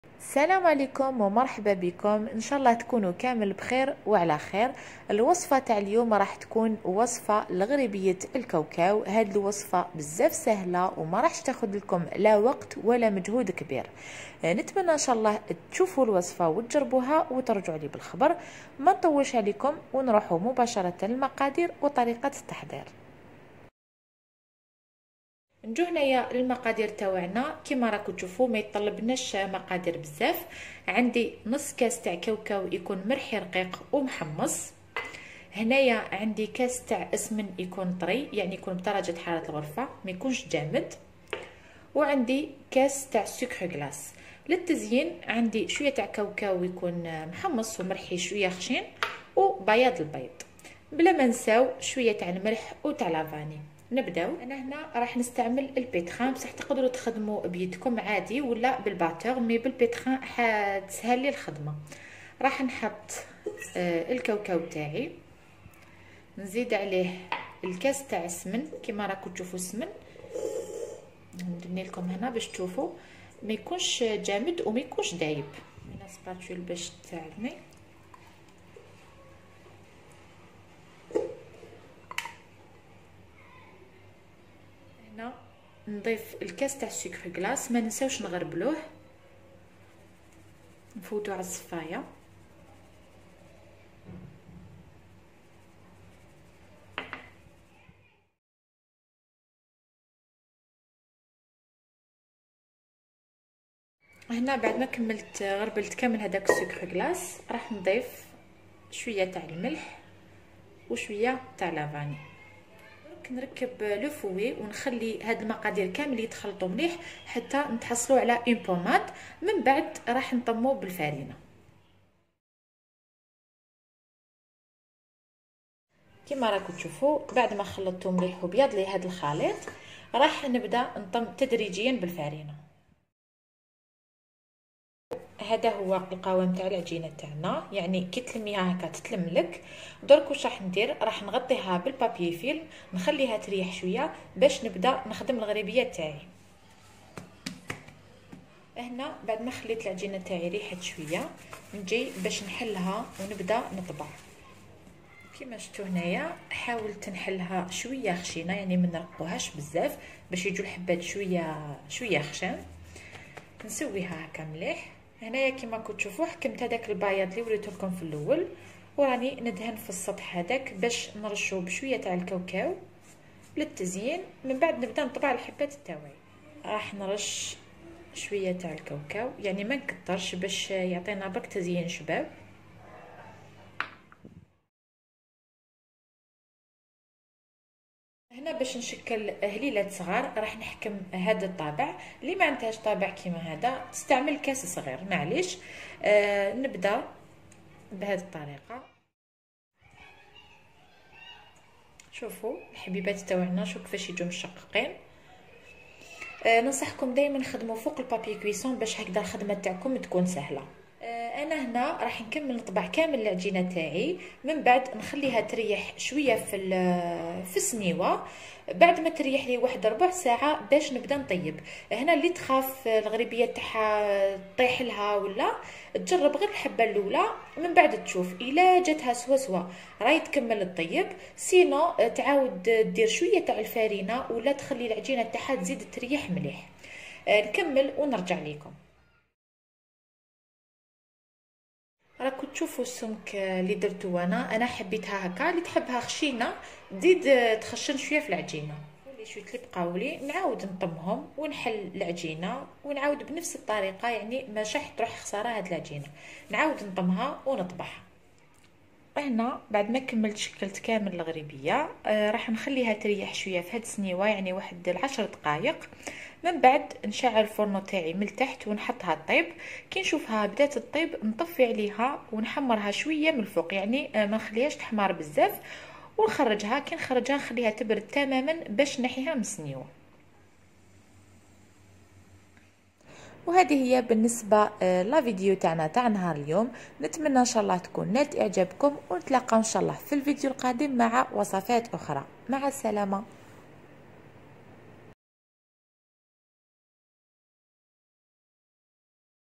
غريبية الكوكاو من اسهل الوصفات 😊
المكونات
كاس سمن/نصف كاس كوكاو محمص ومرحي/كاس سكر ناعم/قرصة ملح/فانيليا /فرينة لجمع العجين